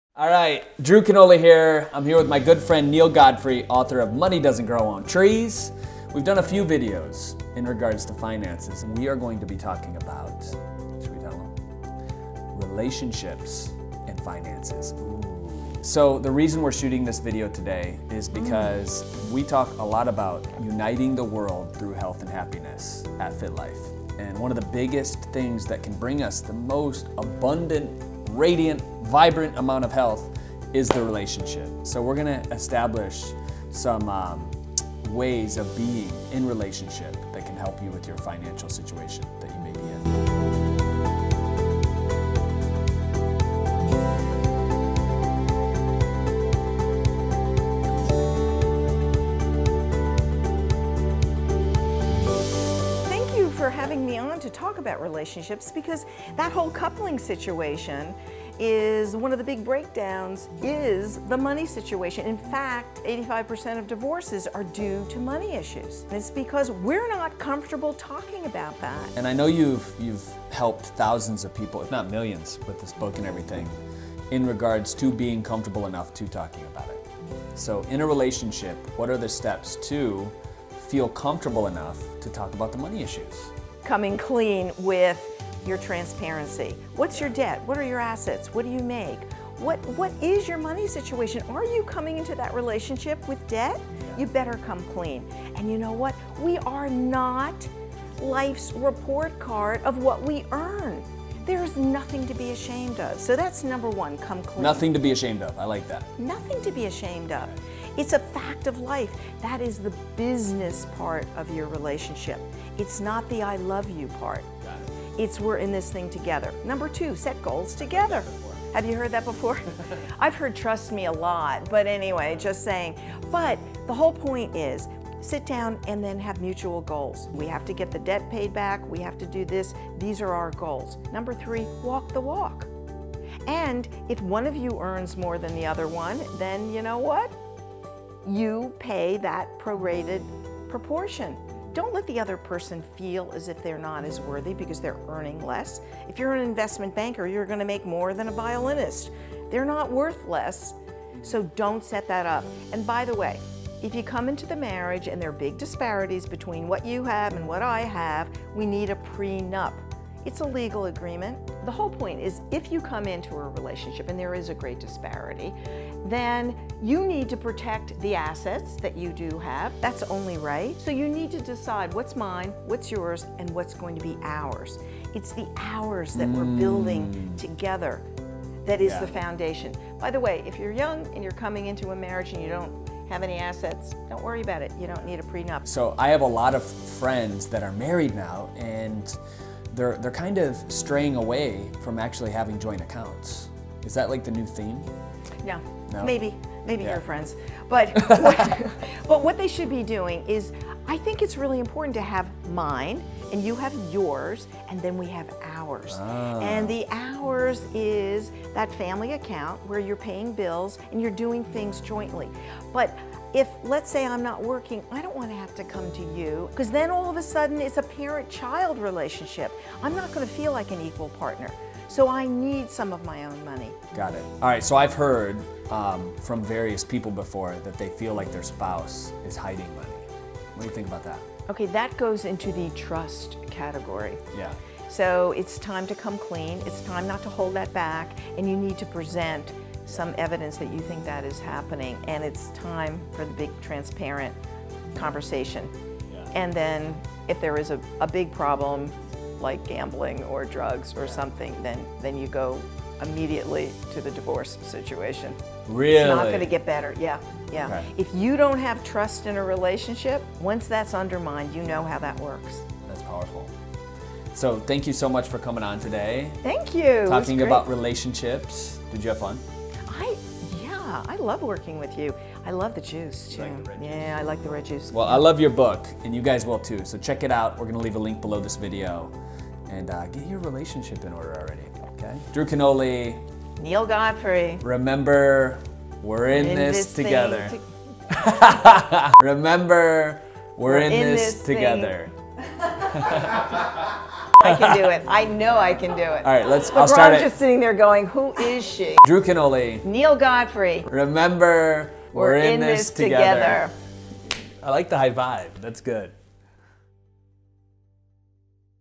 How To Have Healthy Money Habits In Your Relationship
Neale Sheila Godfrey is an American author, acknowledged expert on family and children’s finances and creator of the topic of kids and money in the United States, with over 26 books on financial literacy, intended for both children and adults.

We are so excited to have her here with us today for another Financial Friday blog series!

Finances are an enormous area to navigate in marriage and in other intimate relationships. In fact, according to recent statistics, 85% of divorces are rooted in money problems.

I’ve had plenty of friends come to me complaining about financial issues with their partner. Unfortunately, it is extremely common. However, there are plenty of people who also thrive in their relationships when it comes to money. How do they do it? What makes them different from the rest of us? Let’s dive into some of the best ways to speak about money in relationships:

#1. Come Clean With Your Transparency

One of the biggest reasons couples begin having a hard time with money is because they aren’t completely transparent with each other. From day one of entering a committed relationship, all things money related should be laid on the table.

Debts, assets, resources, income, loans, etc. should all be information that both partners are 100% aware of. There is no good reason to keep anything money related a secret when you are about to begin a life together with someone.

For the rest of Neale’s tips and more information about taking control of your finances, visit the full blog